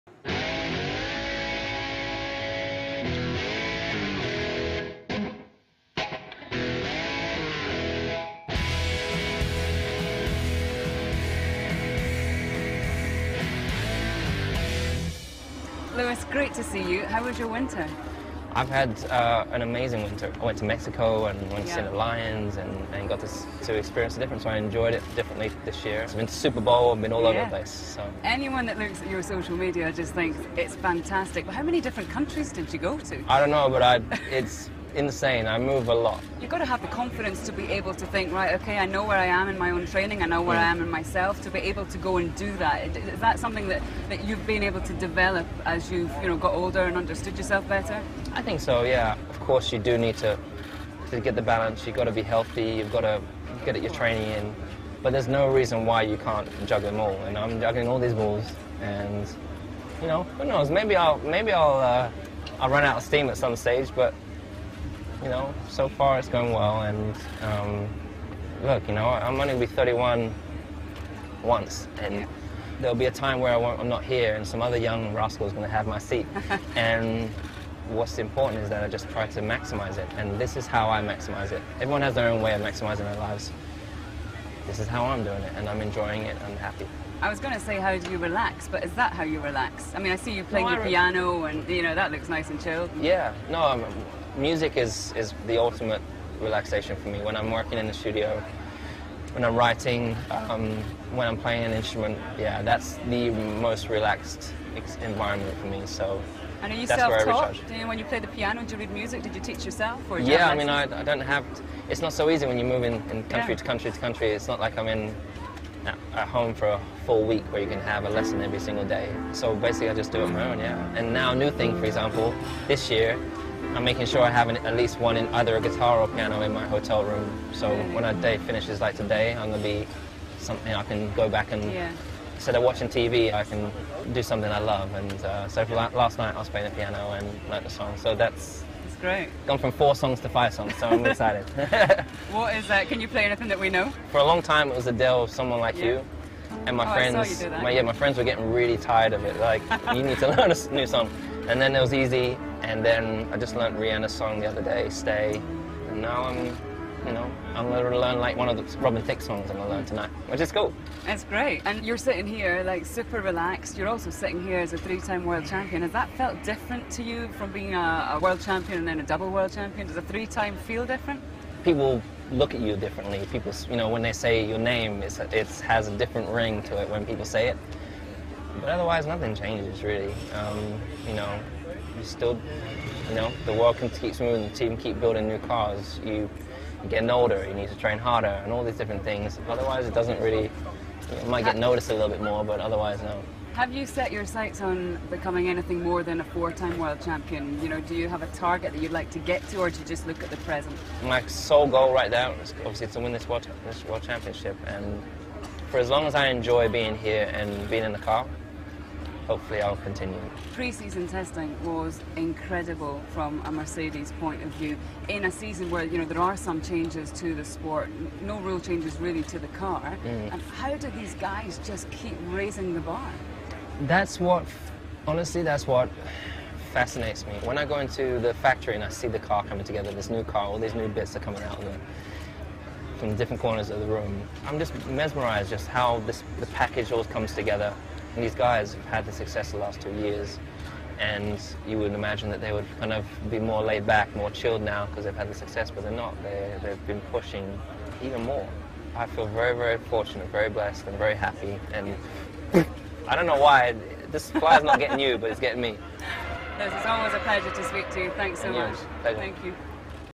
Lewis Hamilton on his lifestyle. Australian GP 2016
Coverage by Channel 4. Interviewer - Lee McKenzie.